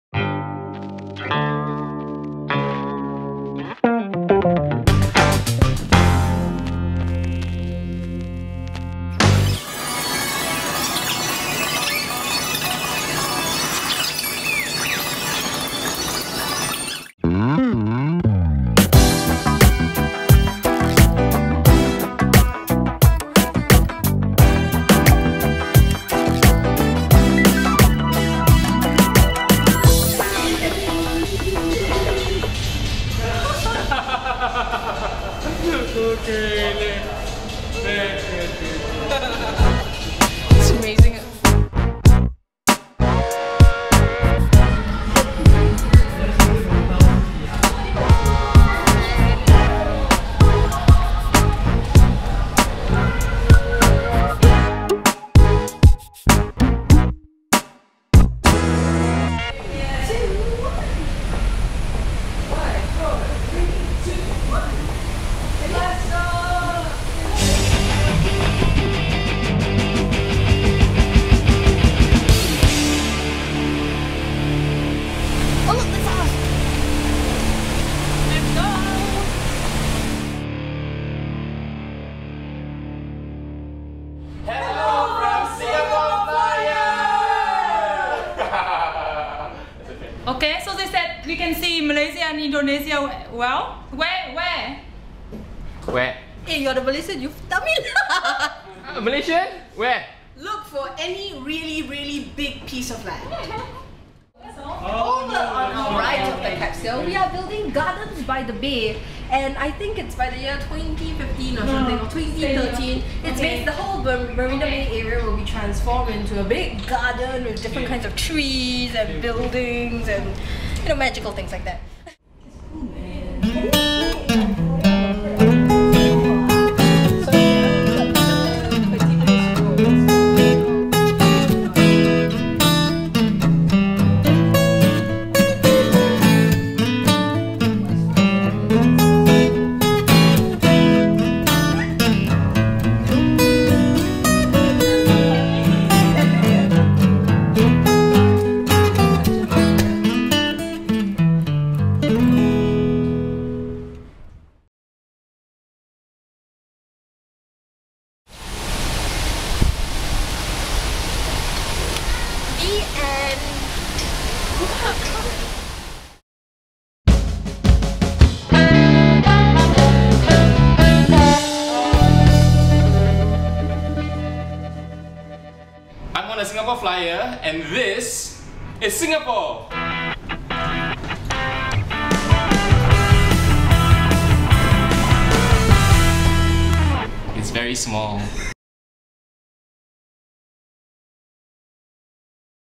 The AJF Fellows take on the Singapore Flyer
Early in April, a few of the Asia Journalism Fellowship, umm, fellows together with a visiting journalist from Malaysia decided to make a trip to the Singapore Flyer. It was quite an experience (we got our own capsule!), and the fun began even wayyyy before we got on the Flyer.

Make sure to watch till after the credits. ;)